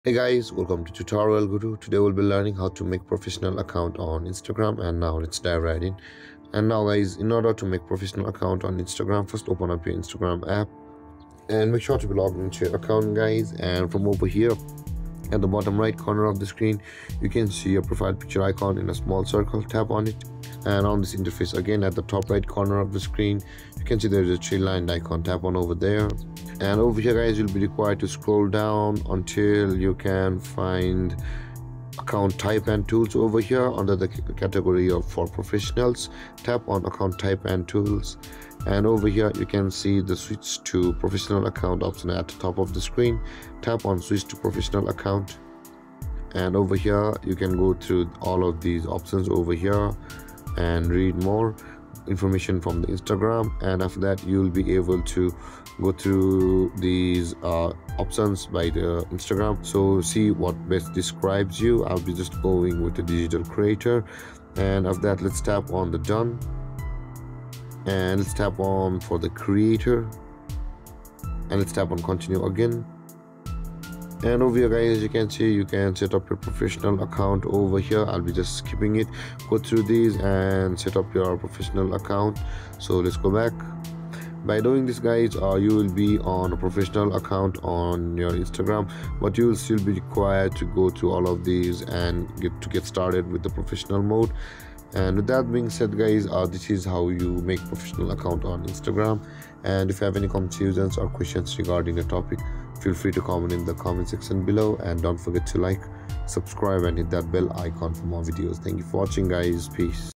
How to Make Professional Account on Instagram?
How to Create a Professional Account on Instagram | TutorialGuru

Welcome to TutorialGuru, your go-to destination for simplified tech tutorials! If you're looking to elevate your presence on Instagram and access advanced features tailored for businesses and creators, creating a professional account is the way to go. In this tutorial, we'll walk you through the steps to set up a professional account on Instagram.